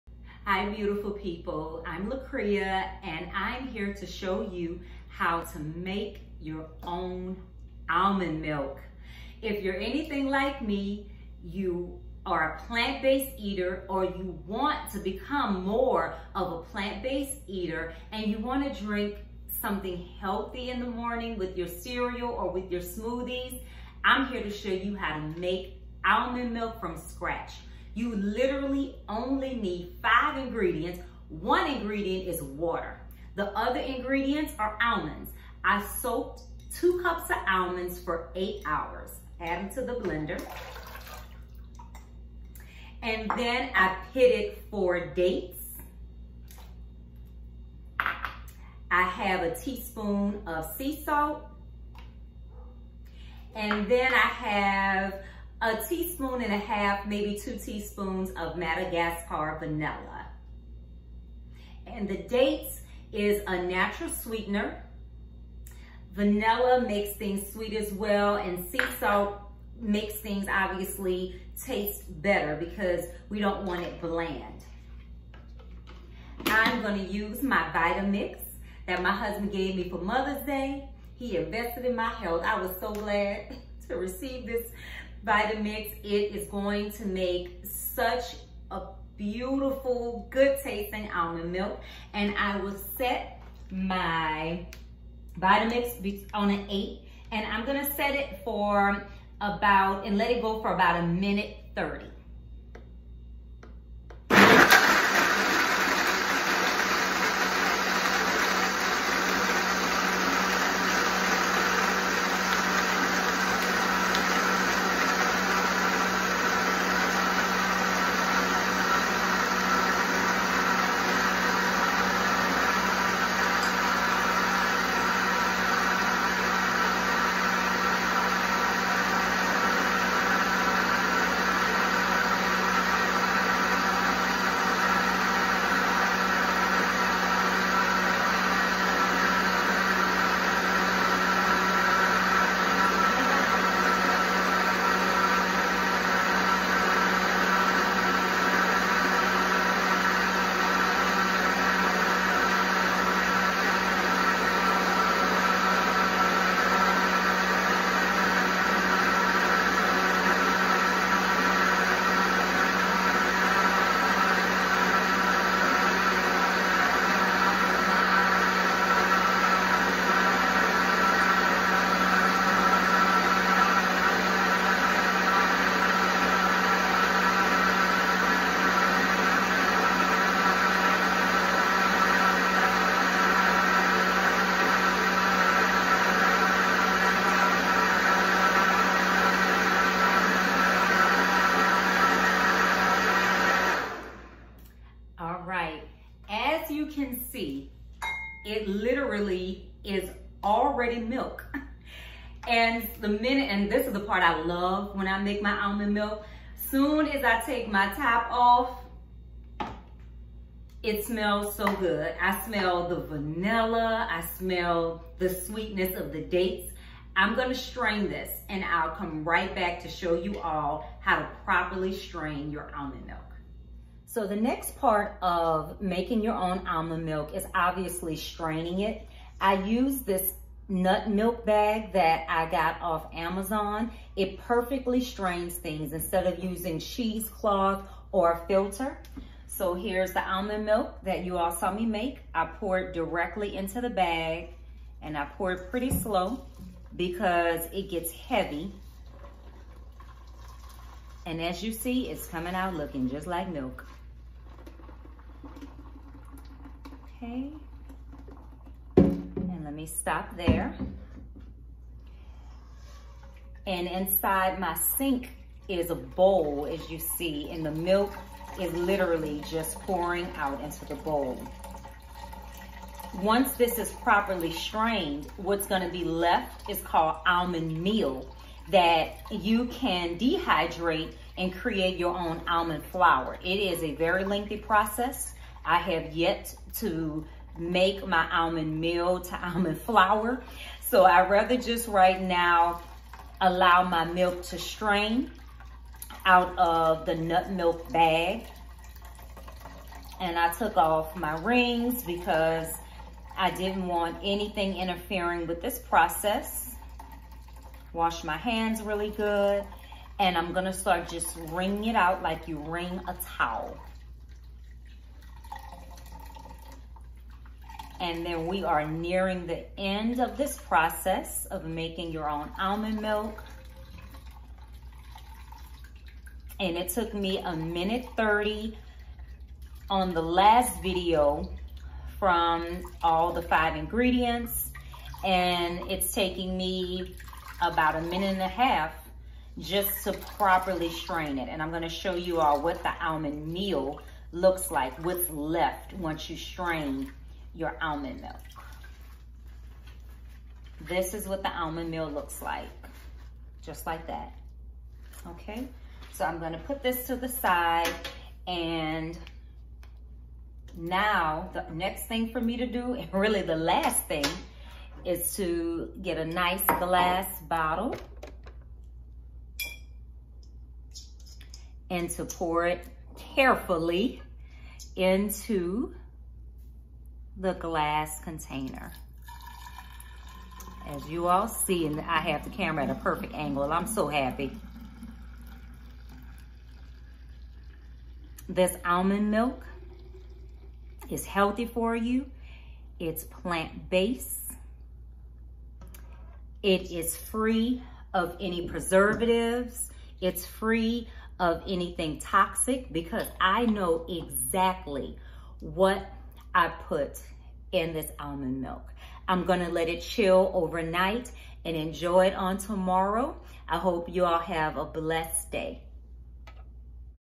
Homemade Almond Milk
Almond Milk
Homemade Almond Milk is nutritious and delicious free of preservatives.
Ingredients
2 cups of soaked almonds in water for 8 hours.
4 Medijool Dates
2 tsp Vanilla
1 tsp Sea Salt
8 cups of Water

Calories: 112kcal | Carbohydrates: 4.2g | Protein: 4.2g | Fat: 9.8g | Saturated Fat: 1g | Sodium: 12mg | Potassium: 252mg | Fiber: 4g | Sugar: 1g | Calcium: 102mg | Iron: 1.3mg